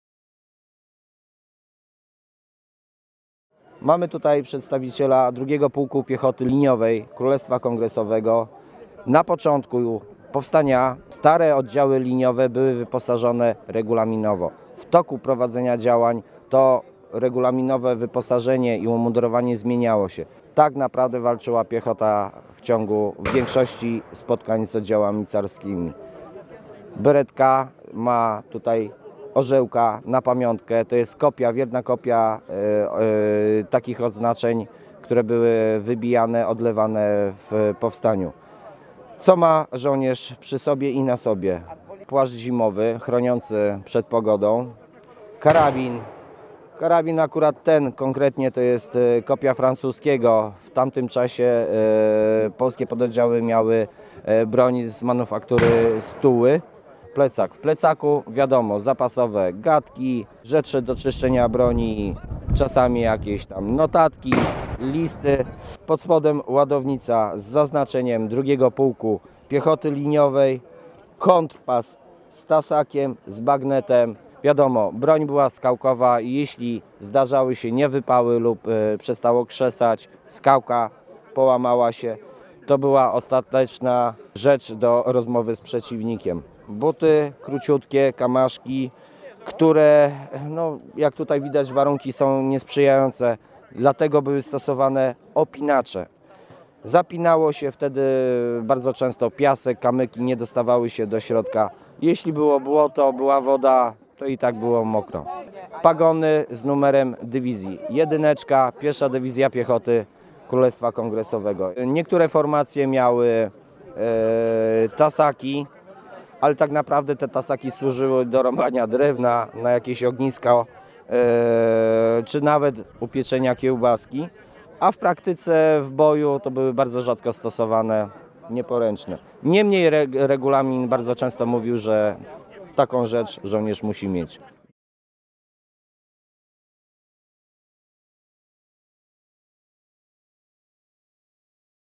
Żołnierz powstania listopadowego
„Na początku powstania listopadowego stare oddziały liniowe były wyposażone regulaminowo. W toku prowadzenia działań to wyposażenie i umundurowanie zmieniało się" -- mówi PAP Dariusz Szpakowski z 2. Pułku Piechoty Księstwa Warszawskiego
Do podstawowych części uzbrojenia piechurów należały: karabin skałkowy, bagnet oraz - używany w różnych celach - tasak. Na wyposażeniu piechura znajdowały się ładownice przytroczone do pasa wojskowego, mundur, furażerka, buty i płaszcz zimowy. Część ekwipunku noszono w plecaku.